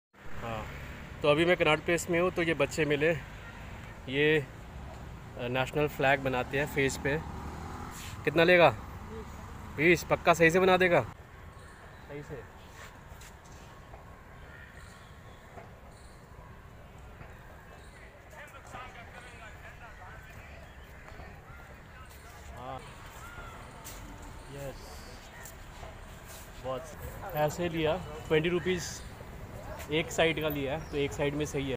Street Artist🤩/ / /
Hello dosto...🤩🤩🤩🇮🇳🇮🇳🇮🇳🇮🇳

Ye video Delhi key Connaught place palika bazar gate no 2 k samne banaya hai jaha her saal asey he alag alag design banaya jata hai .🇮🇳🇮🇳🇮🇳🇮🇳

 1 - Ye mera channel name hai 😀

2 - Ye mera YouTube channel link hai 😊

youtube shorts
shorts